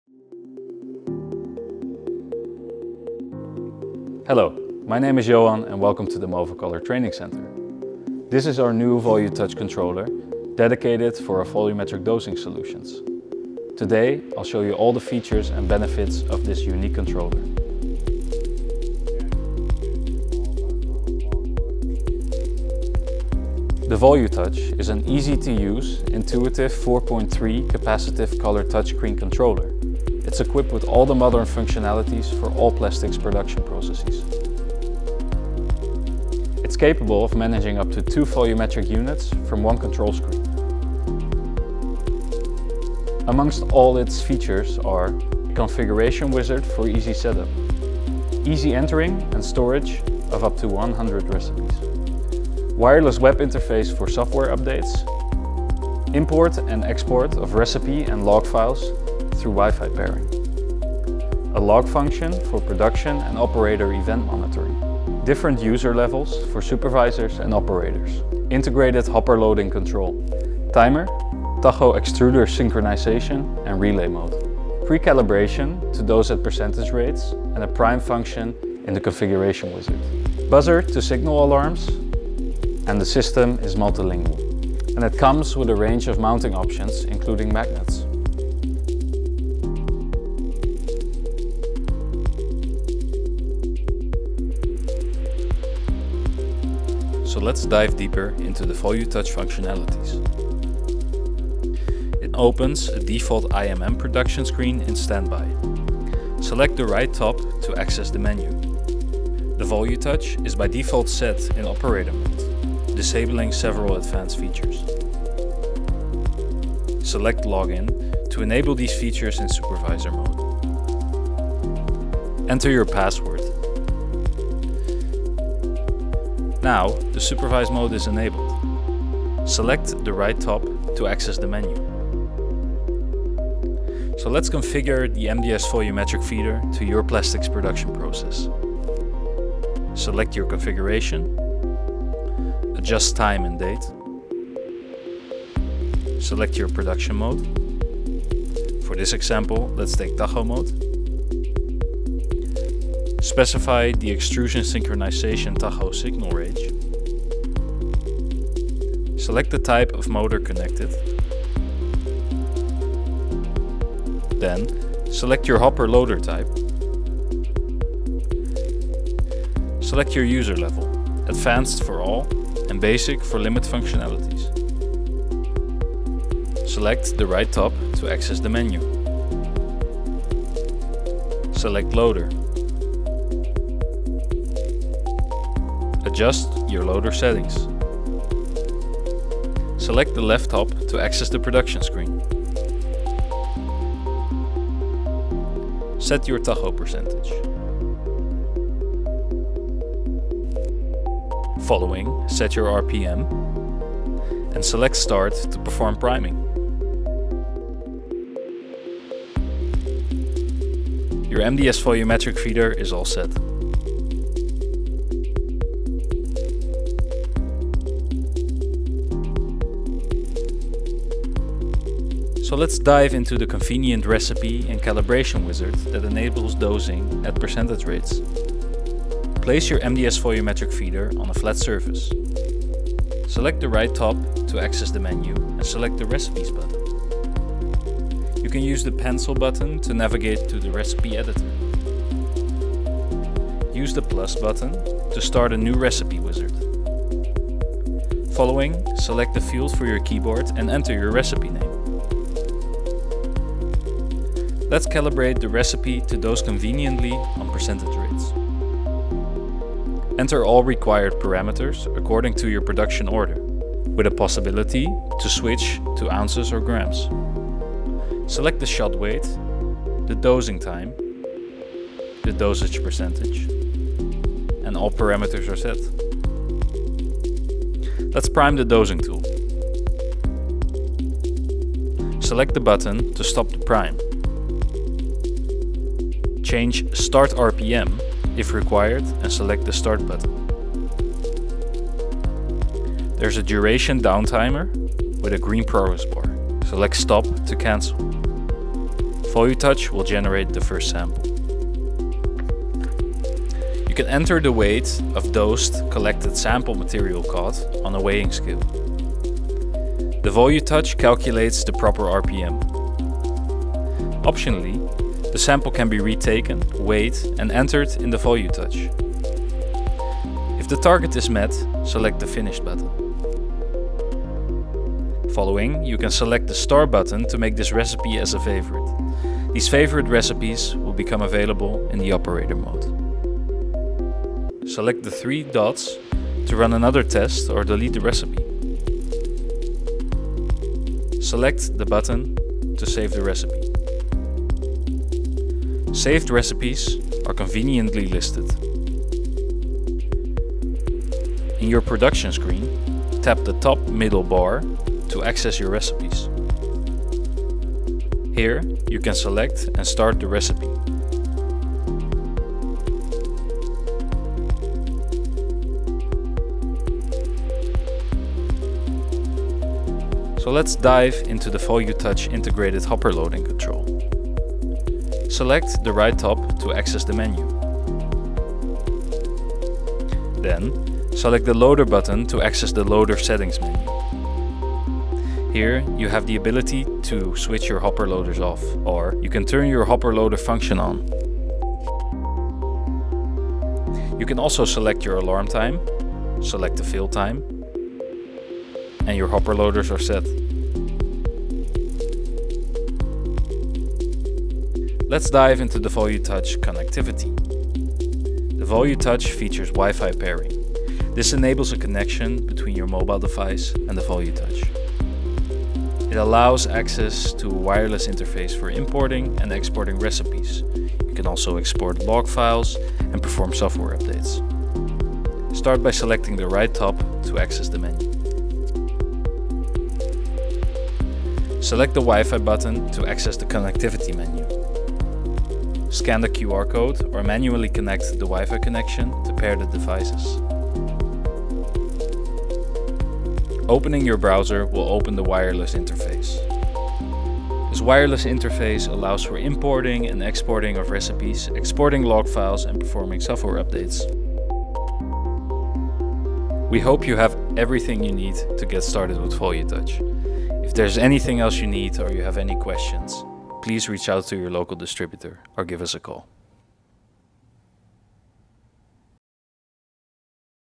The User Interface of the MDS Volumetric Feeder
Explore the user interface features of the MDS Volumetric Feeder. Do you want to discover all the benefits of our new volumetric dosing solution?